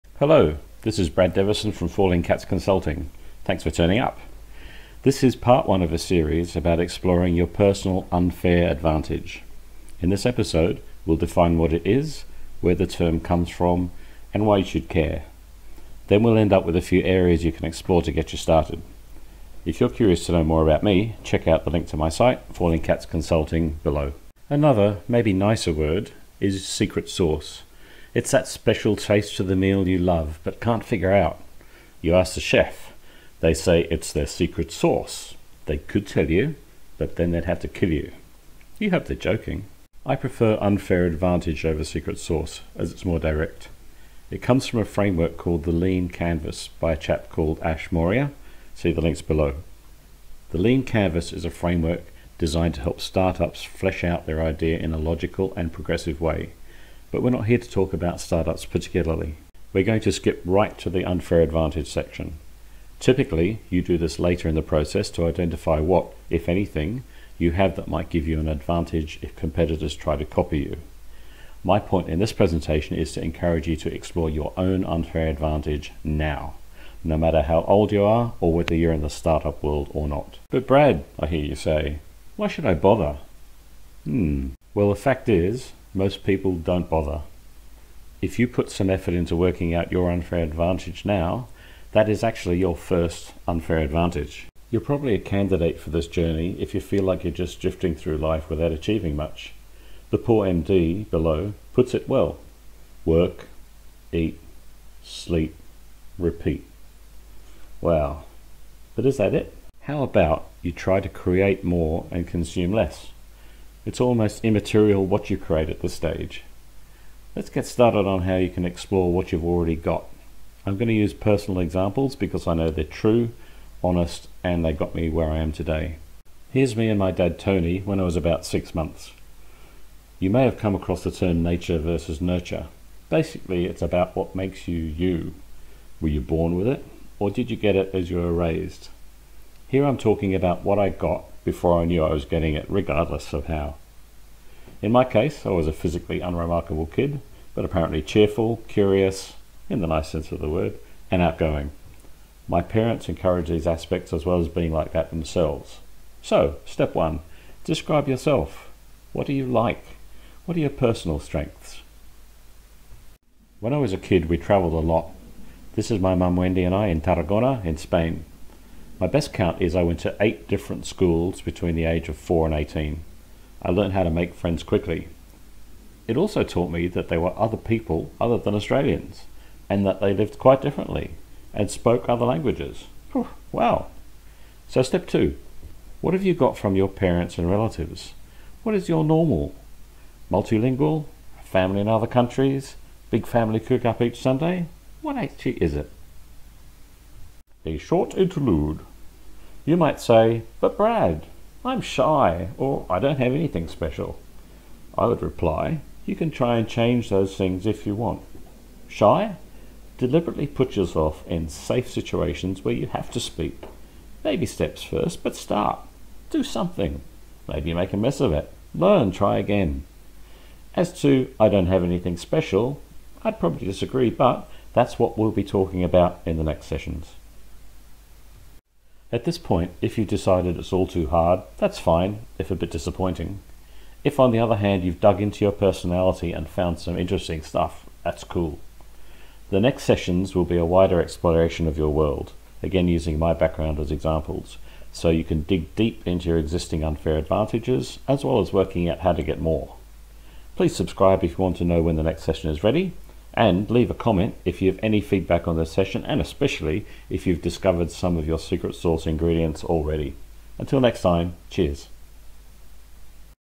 Exploring your Unfair Advantage - part 1
This is the first of a series aimed at helping anyone work out what their Unfair Advantage is, as well as to discover more to work on. In this context an Unfair Advantage is something that is very specific to you, that gives you a kick start in life.
This first episode defines Unfair Advantage, gives its origins and starts with a personal audit of you as a person. Right now! Ready?